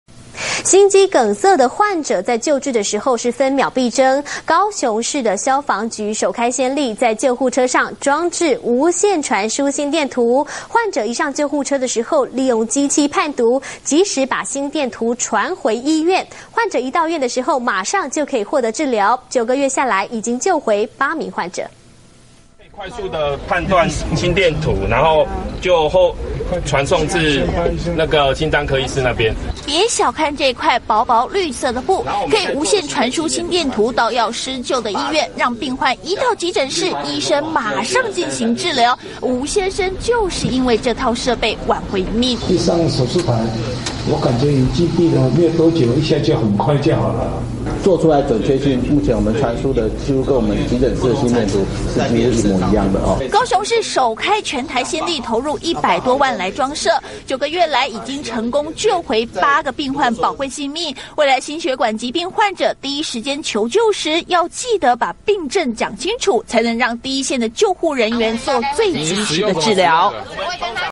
中視新聞》搶時救人！ 救護車裝心電圖 已救回8命
內容轉載自《中時電子報》：
中視新聞》心肌梗塞患者發病時分秒必爭，高雄市消防局首開先例，在救護車上裝置無線傳輸心電圖，患者一上救護車，利用機器判讀，及時把心電圖傳給醫院，患者到院馬上可以獲得治療，九個月來已經救回八名病患寶貴性命。